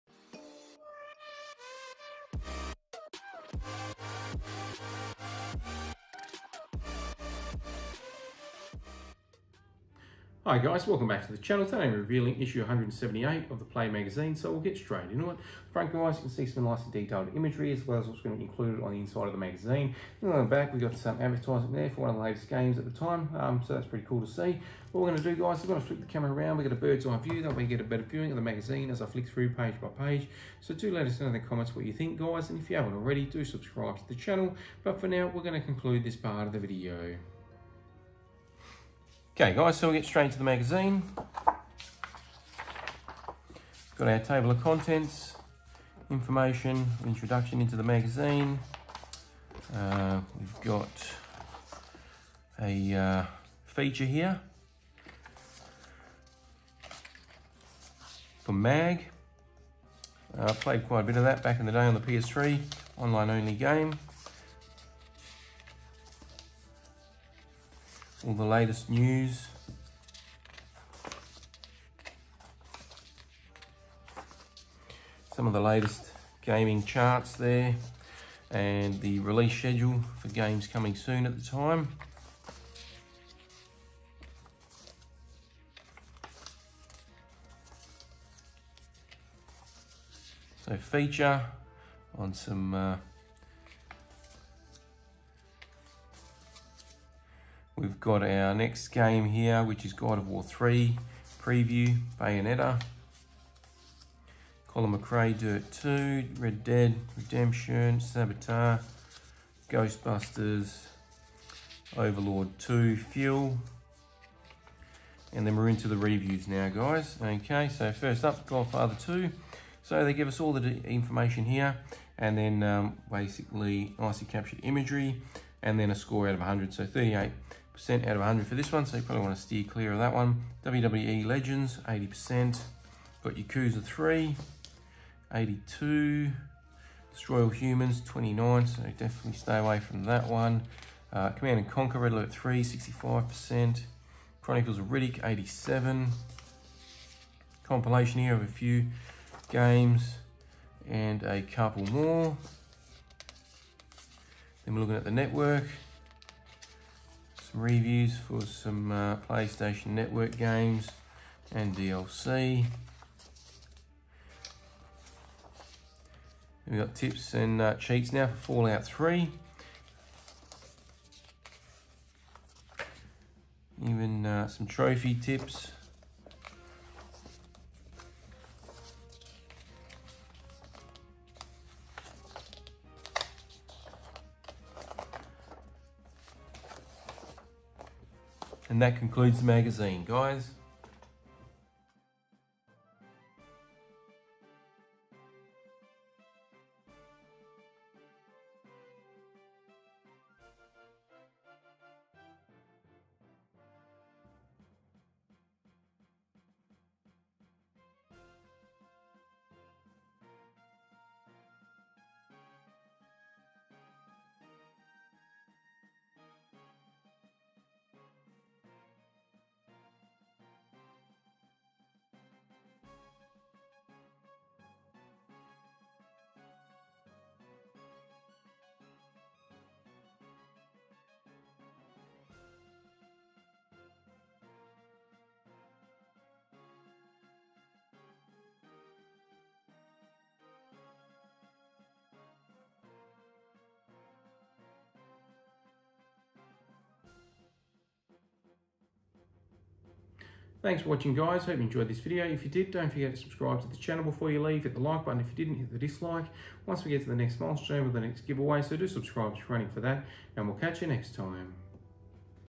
Ep 610 - Play Issue 178 Magazine Revealing
Hi Guys

Welcome to the channel.  Please enjoy my revealing video of the Play Magazine.

Fan Mail -

Australia 6906